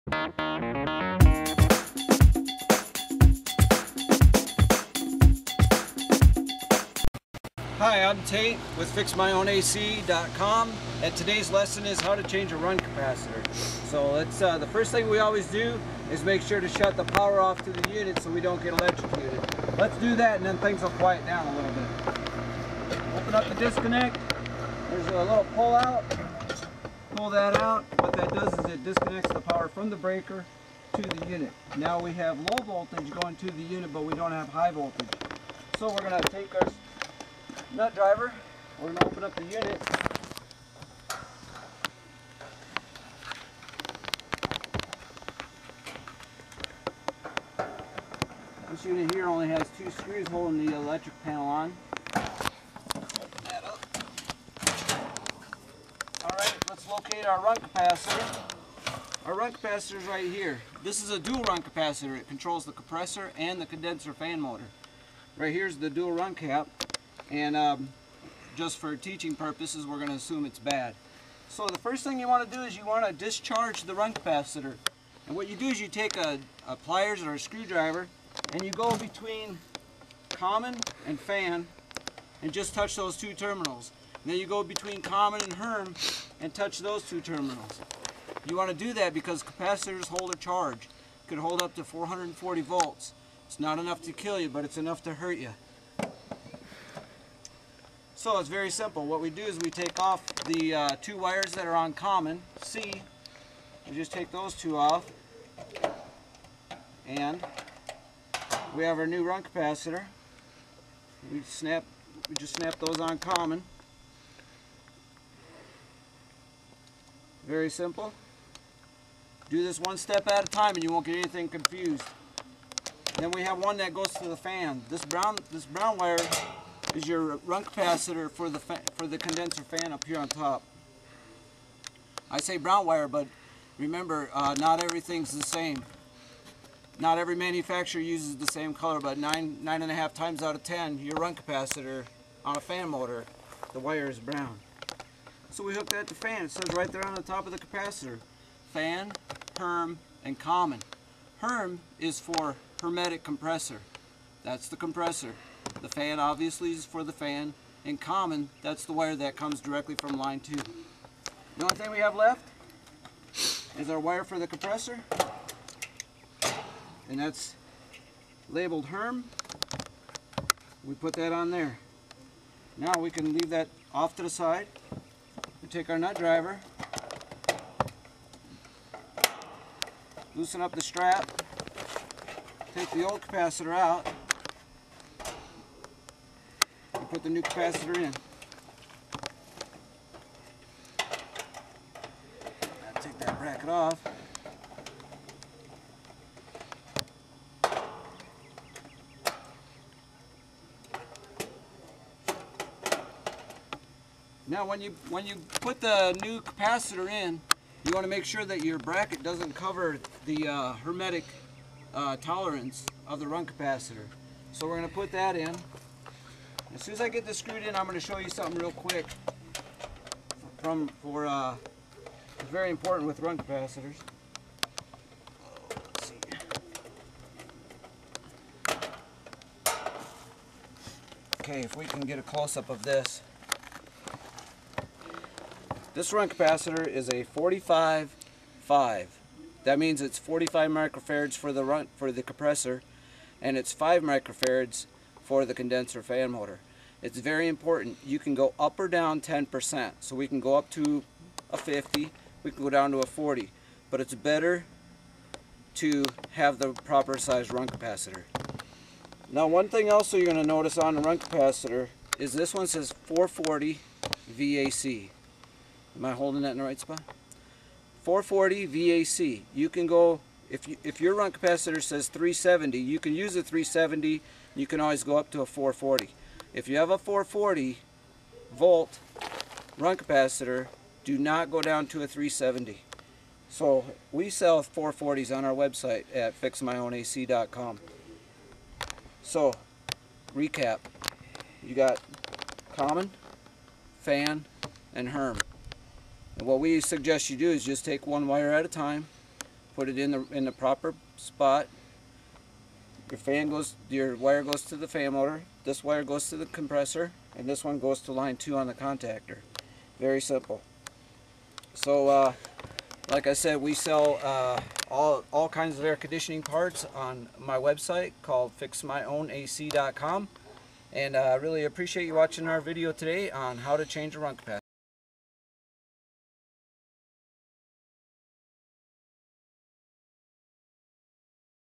Fix My Own AC - How to Change a Run Capacitor
- Learn how to change a run capacitor in your air conditioner unit. Fix My Own AC accepts no responsibility for damages to you or your property. All repairs are at your own risk.